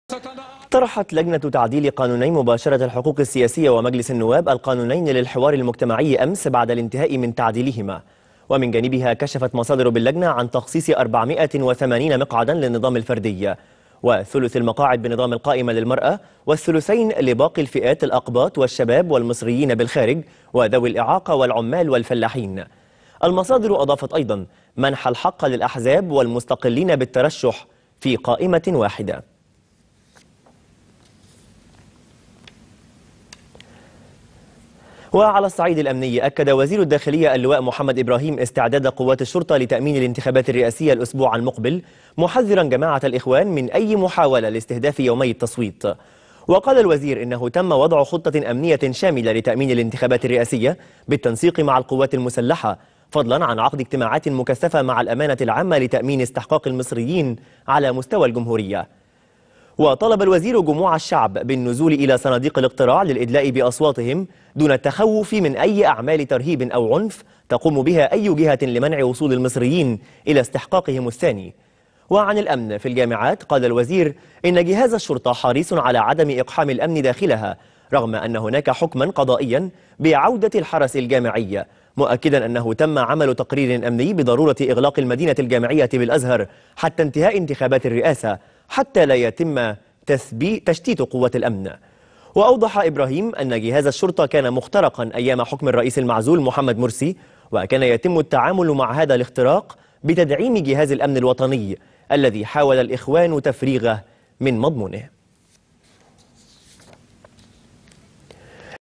طرحت لجنةُ تعديلِ قانوني مباشرةِ الحقوقِ السياسيةِ ومجلسِ النواب القانونينِ للحوارِ المجتمعيِ
طرحت لجنةُ تعديلِ قانوني مباشرةِ الحقوقِ السياسيةِ ومجلسِ النواب القانونينِ للحوارِ المجتمعيِ أمسِ بعدَ الانتهاءِ من تعديلِهما

أكد وزيرُ الداخلية اللواء محمد إبراهيم استعدادَ قواتِ الشرطة لتأمينِ الانتخابات الرئاسية الأسبوعَ المقبل محذرا جماعةَ الإخوان من أي محاولة لاستهدافِ يومي التصويت